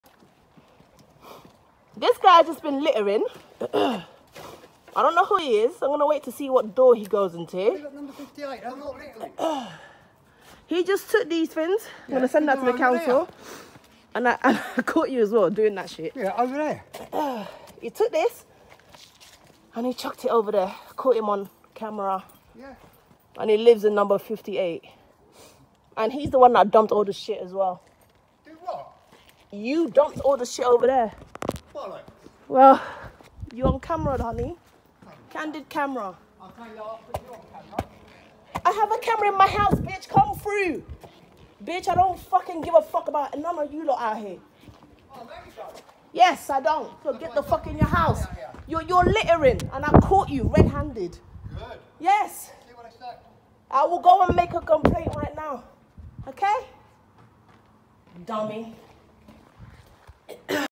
Disgusting Litter Bug…Caught Red Handed - Male Karen
And the Dummy admitted it too!…He was too shook..he went and picked his trash back up after he was caught on Cam..
He’s won DONKEY OF THE CENTURY